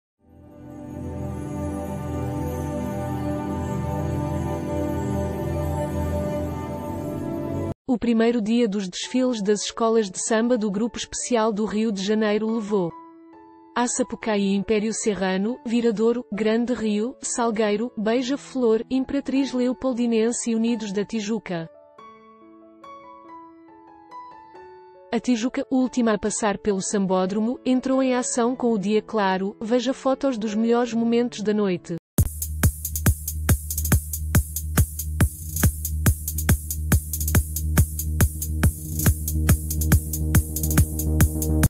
Veja fotos do desfile das escolas de samba do Rio
O primeiro dia dos desfiles das escolas de samba do Grupo Especial do Rio de Janeiro levou à Sapucaí...